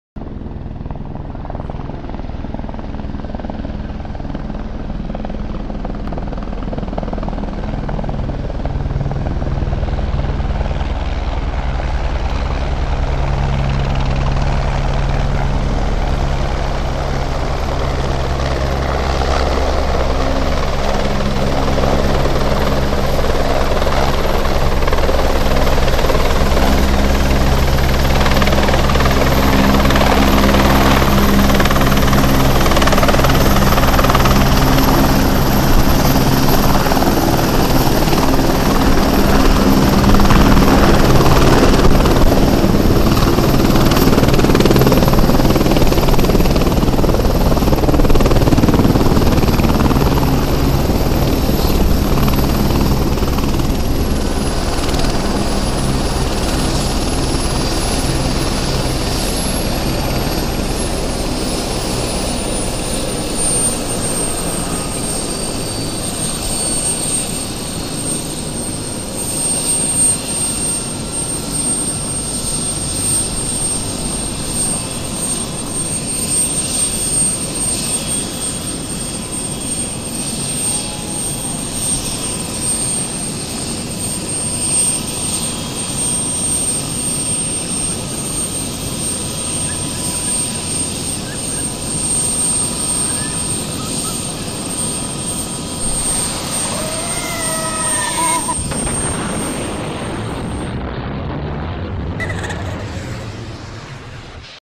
Benedict Cumberbatch Helicopter at Excel
Benedict Cumberbatch arriving at Sherlocked Excel